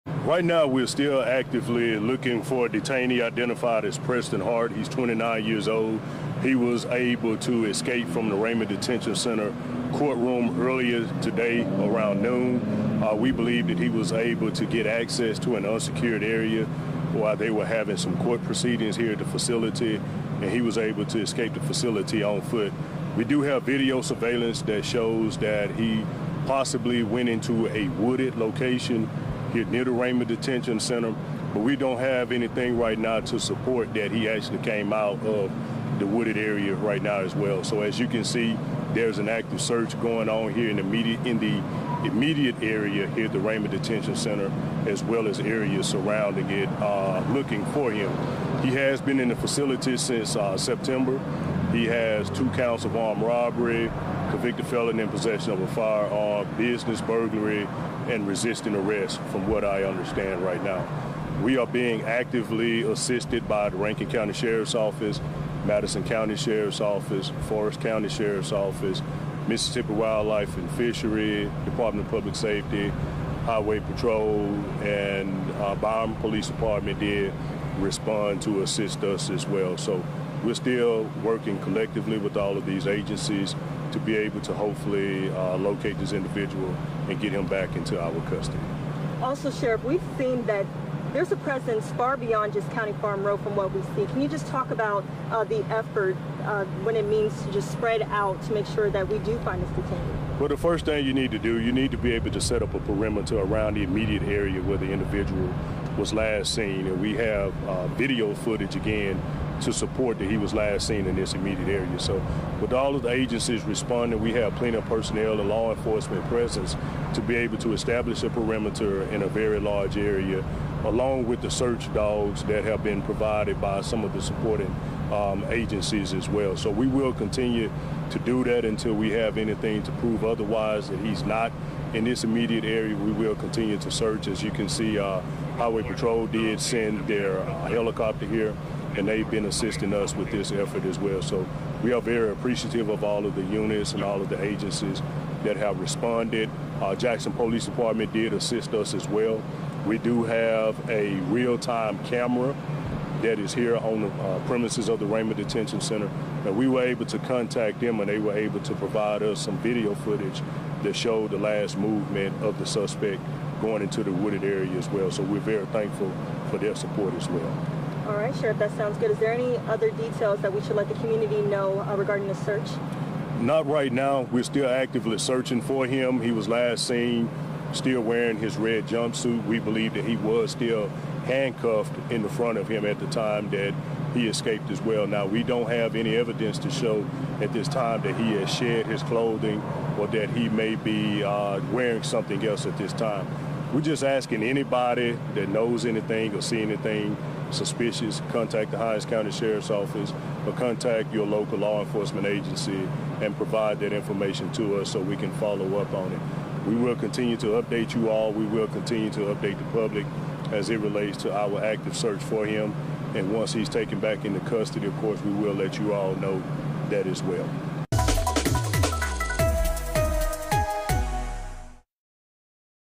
Hinds County Sheriff's Office searching for escaped detainee
Hinds County Sheriff Tyree Jones explains how a detainee was able to escape from the Raymond Detention Center.